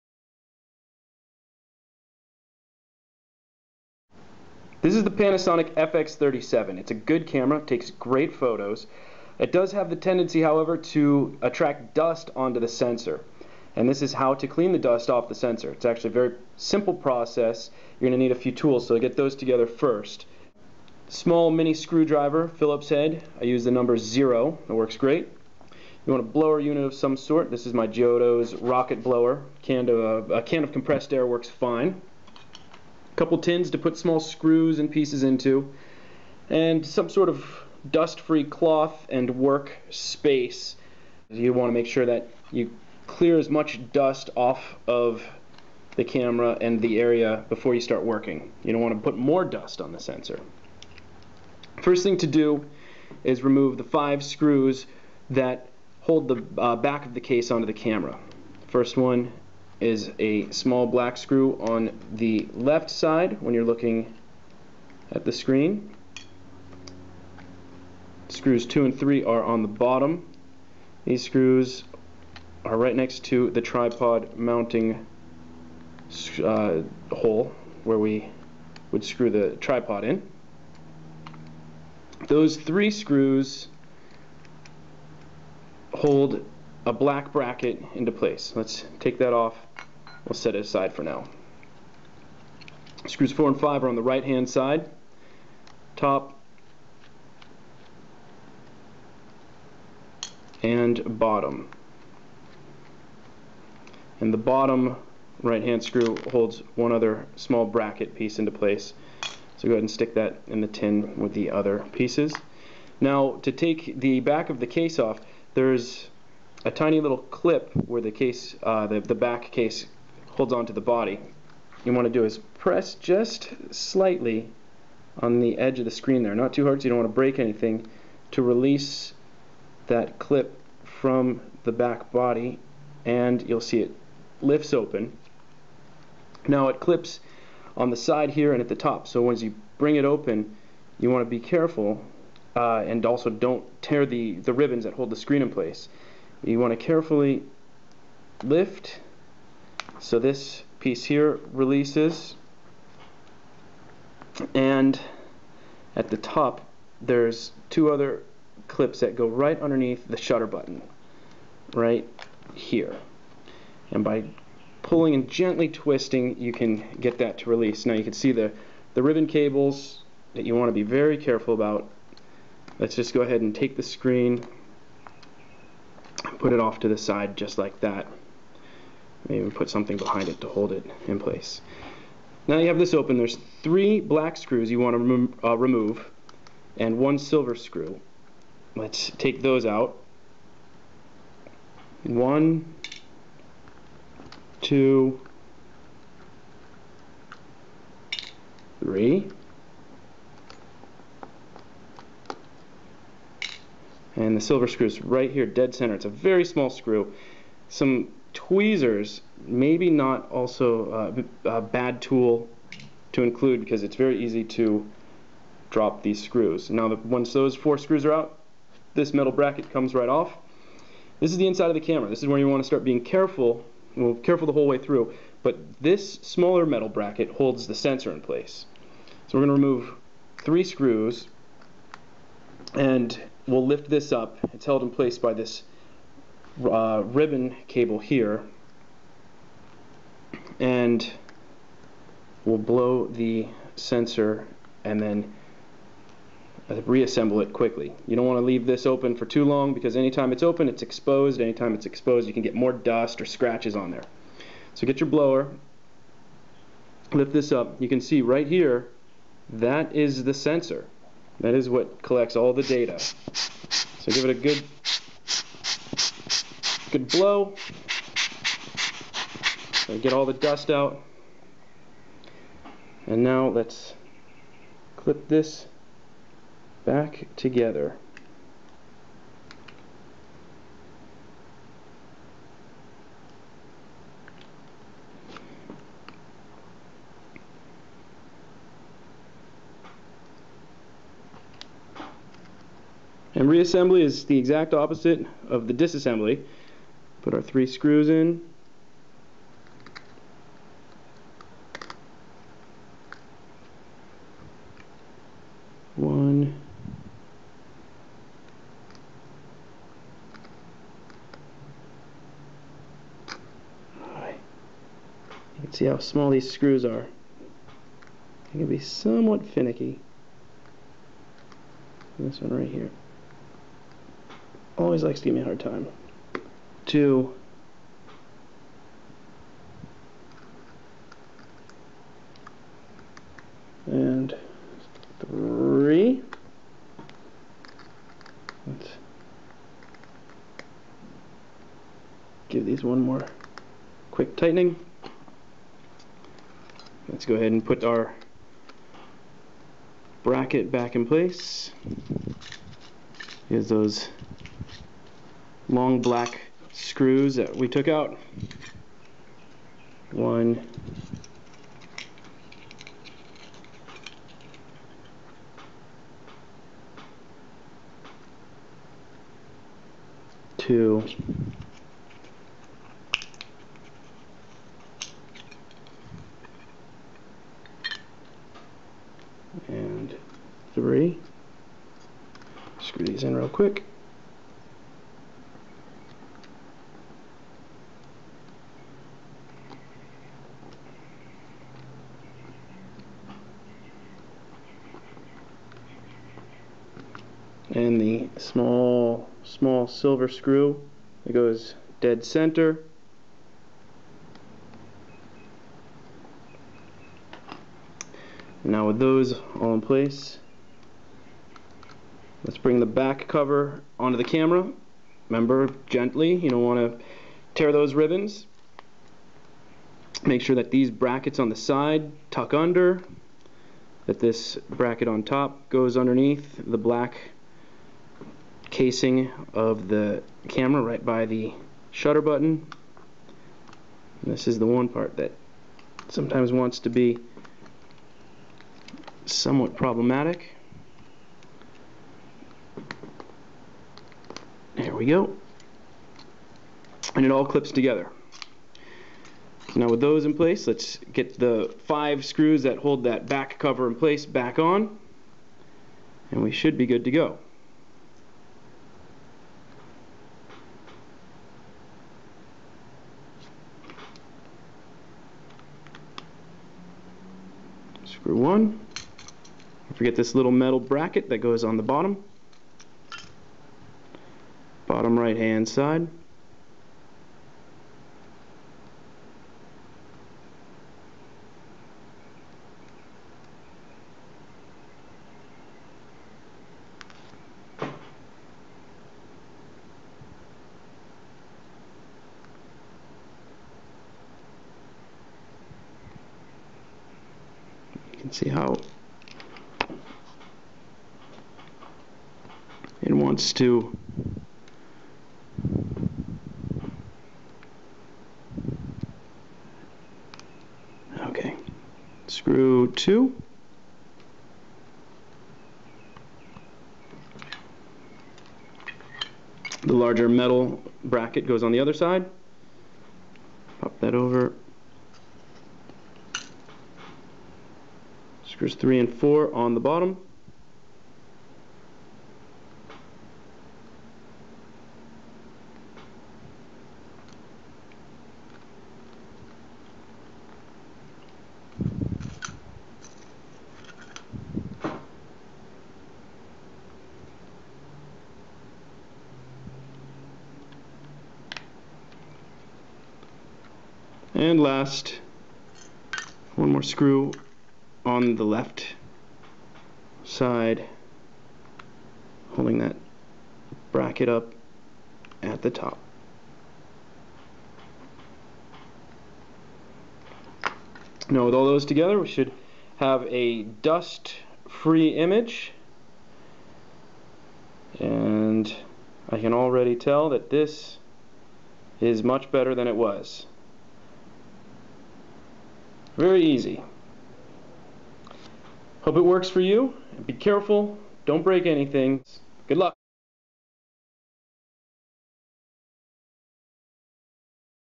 Surprisingly Easy Sensor Cleaning and Dust Removal: Panasonic Lumix FX37 Digital Camera
Be careful disassembling your camera. This will almost surely void your warranty, so be certain that you are willing to risk breaking your camera. I take no responsibility for any damage incurred following these instructions.